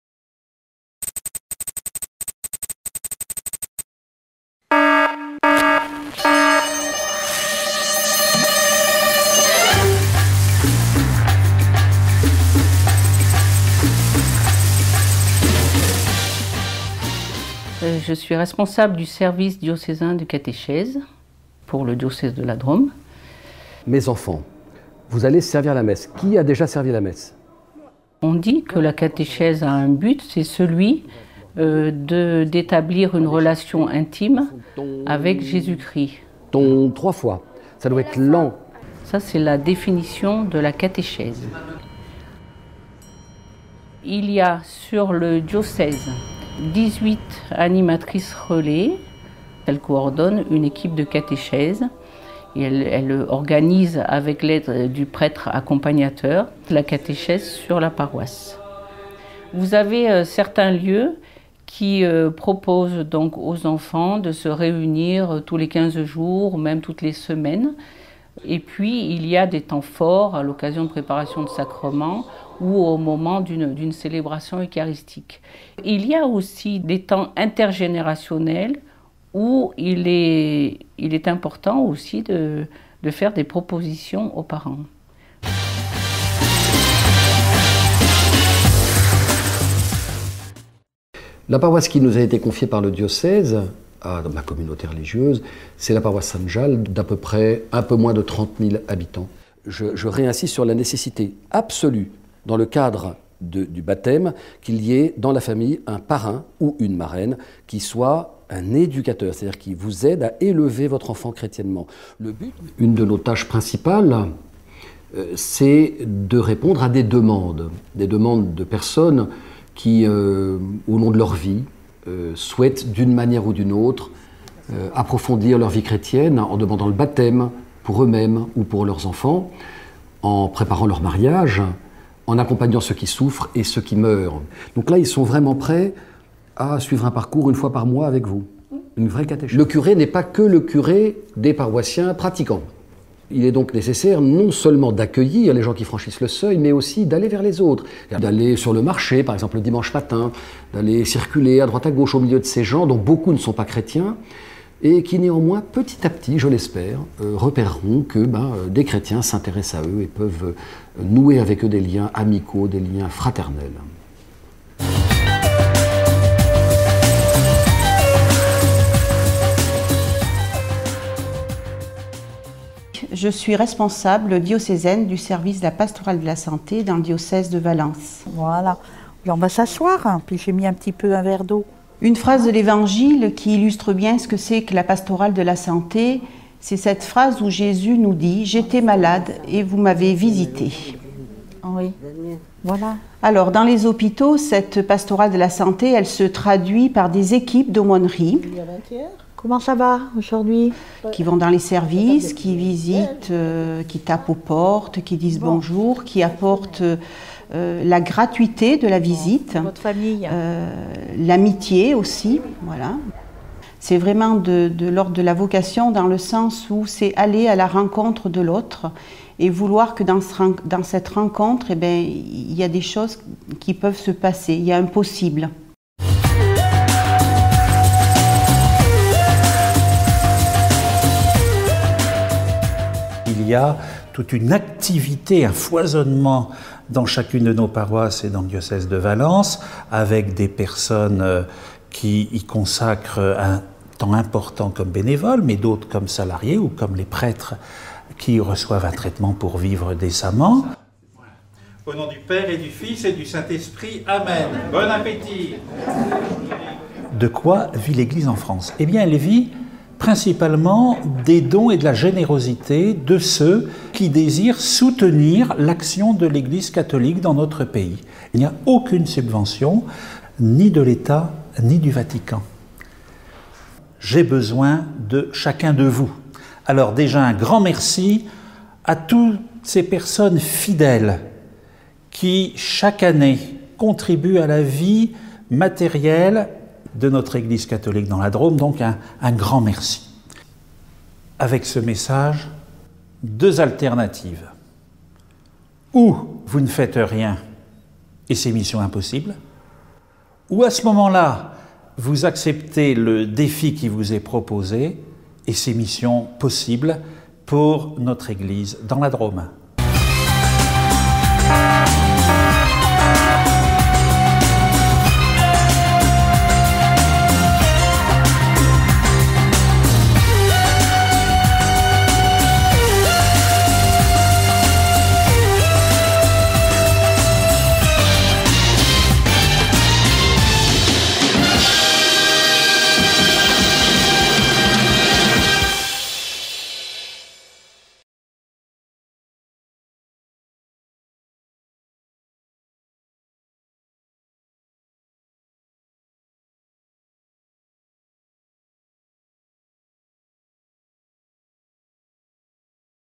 Denier de l'Eglise 2011, Diocèse de Valence
La collecte du Denier, lancée le 17 AVRIL 2011, le jour des Rameaux, permet de couvrir les traitements et les salaires des personnes au service du diocèse de Valence. C'est la principale ressource de notre Eglise de la Drôme.